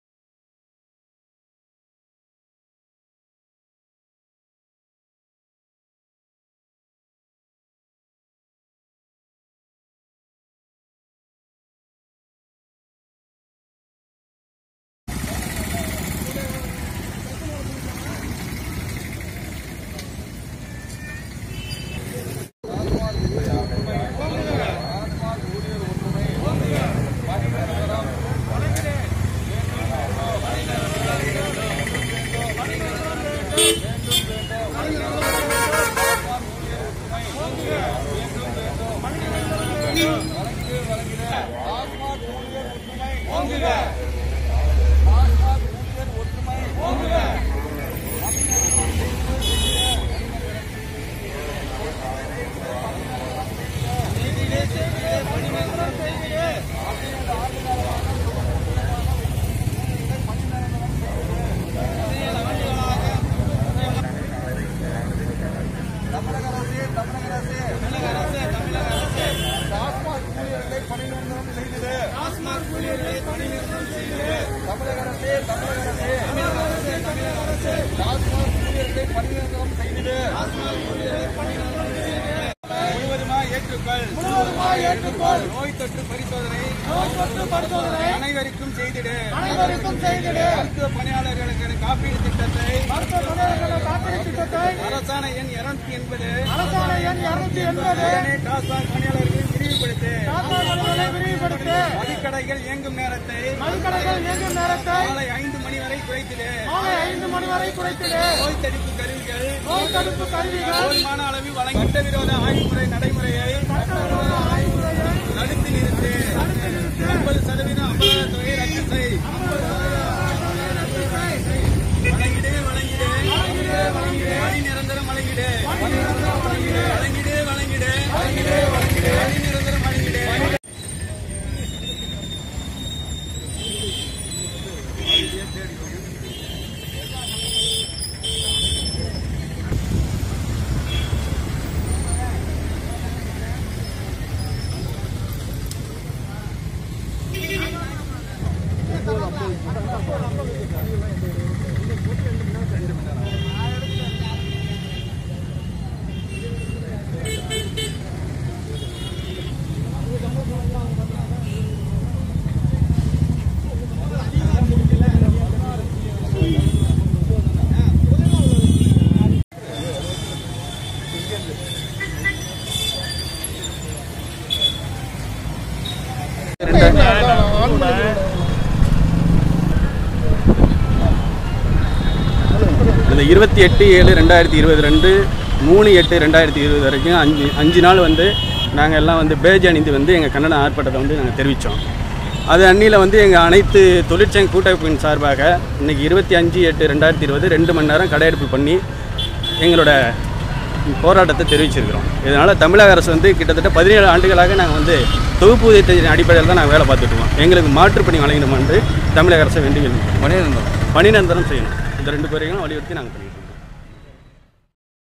Kumbakonam News Aug 2020 Tasmac Staff Two Hours Strike 28 Shops Closed & Demo on 25 08 20
17 ஆண்டுகள் பணியாற்றும், டாஸ்மாக் ஊழியர்களை தமிழக அரசு   பணி நிரந்தரம் செய்ய வேண்டும் என்பது உள்ளிட்ட கோரிக்கைகளை முன்வைத்து, கும்பகோணத்தில் டாஸ்மாக் ஊழியர்கள் இரண்டு மணி நேரம் வேலை நிறுத்தம் செய்து, கடை முன் திரண்டு ஆர்ப்பாட்டம், இதனால் கும்பகோணத்தில் காலை 10 மணி முதல் மதியம் 12 வரை 28 டாஸ்மாக் கடைகளும் அடைக்கப்பட்டிருந்தது

   மூன்று ஆண்டுகள் ஆனாலே பணி நிரந்தரம் செய்ய வேண்டும் என்ற அரசு விதிக்கு எதிராக, 17 ஆண்டுகள் ஆன பின்பும் இதுவரை டாஸ்மாக் ஊழியர்களை பணி நிரந்தரம் செய்ய தமிழக அரசு முன்வராததை கண்டித்தும், உடனடியாக செய்ய வலியுறுத்தியும், கொரோனா தொற்று காலத்தில் பணியாற்றும் டாஸ்மாக் பணியாளர்களுக்கு ரூபாய் 10 ஆயிரம் கூடுதலாக சிறப்பு ஊதியமாக வழங்க வேண்டும், கொரோனாவால் உயிரிழந்த டாஸ்மாக் ஊழியர்களுக்கு ரூபாய் 50 லட்சம் இழப்பீடு வழங்கிடவும், நோய் தொற்று பரிசோதனை ஊழியர்கள் அனைவருக்கும் செய்திடவும், பணியாளர் மற்றும் அவரது குடும்பத்தினர் மருத்துவ செலவினை அரசே ஏற்றுக்கொள்ளவும், மதுகடைகள் இயங்கும் நேரத்தை மாலை 5 மணியாக குறைக்க வேண்டும், நோய் தடுப்பு கருவிகளை போதுமான அளவிற்கு வழங்கிடு, வாரிசிற்கு அரசு பணி வழங்கிட வேண்டும், சட்டவிரோத ஆய்வு முறையை தடுத்து நிறுத்து என்பன போன்ற கோரிக்கைகளை வலியுறுத்தி இன்று மாநிலம் தழுவிய அளவில் 2 மணி நேர வேலை நிறுத்த போராட்டம் நடைபெறுகிறது

    அதன் ஒருபகுதியாக இன்று கும்பகோணத்தில் உள்ள அனைத்து (28) மதுகடைகளும் காலை 10 மணி முதல் மதியம் 12 மணி வரை அடைக்கப்பட்டிருந்தது, ஏராளமான டாஸ்மாக் ஊழியர்கள் காமராஜர் சாலை முன்பு உள்ள கடை எண் 7822 மற்றும் புதிய பேருந்து நிலையம் அருகே உள்ள கடை எண் 7812 ஆகிய கடைகள் முன்பு திரண்டு கோரிக்கை முழக்க கண்டன ஆர்ப்பாட்டத்திலும் ஈடுபட்டனர்

    பேட்டி : சார்லஸ், தஞ்சை மாவட்ட துணை தலைவர்,
           தமிழ்நாடு டாஸ்மாக் ஊழியர்கள் சங்கம்